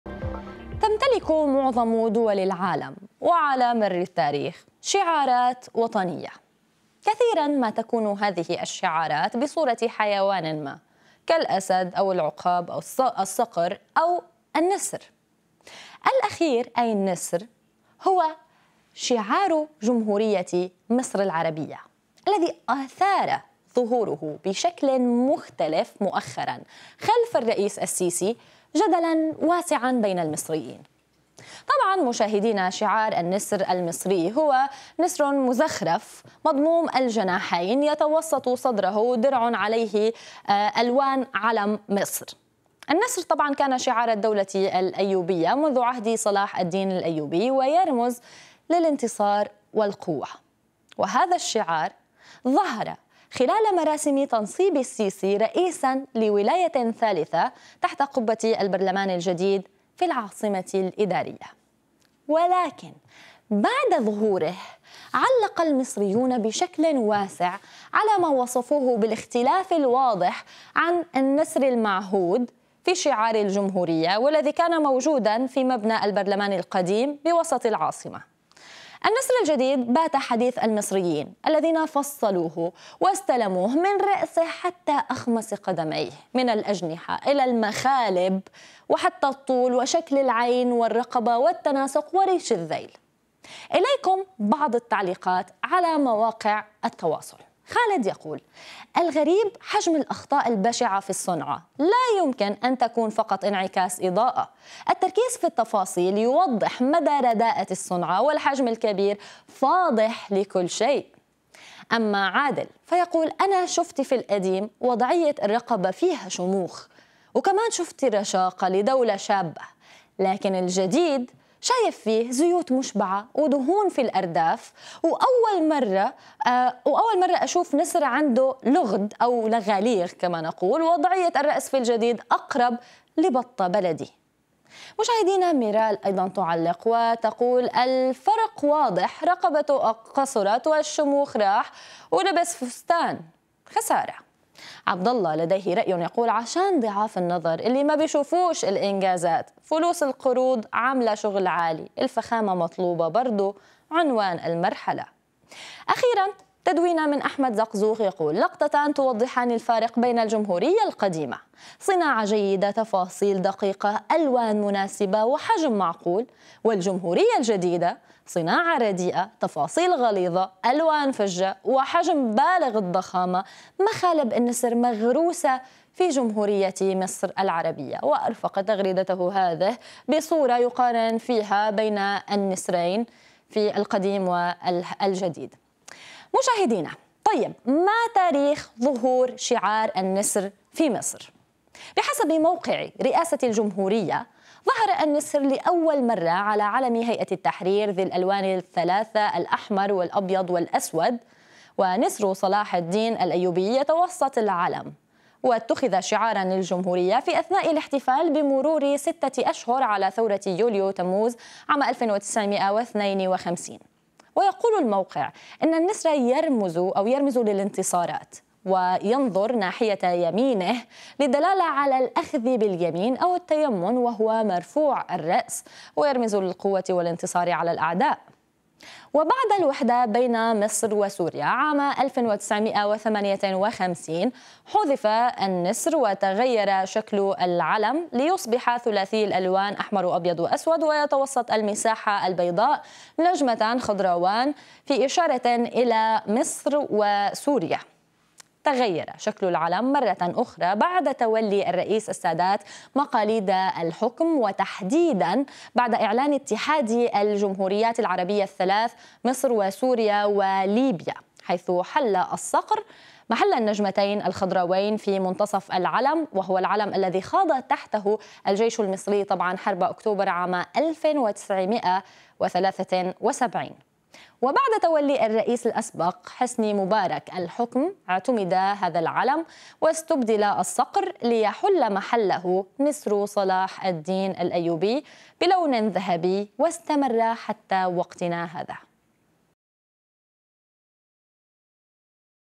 تندر المصريين على شعار ‘’النسر المصري’’ في مبنى البرلمان الجديد بالعاصمة الإدارية
ضجت مواقع التواصل في مصر بصور شعار النسر المصري، عقب مراسم تنصيب الرئيس المصري عبد الفتاح السيسي لولايته الثالثة.

وكانت تلك المراسم أقيمت في العاصمة الإدارية الجديدة، حيث ظهر شعار النسر خلفه تحت قبة البرلمان المصري الجديد.
وبعد ظهوره، علق المصريون بشكل واسع على ما وصفوه بالاختلاف الواضح عن النسر المعهود في شعار الجمهورية والذي كان موجودا في مبنى البرلمان القديم بوسط القاهرة.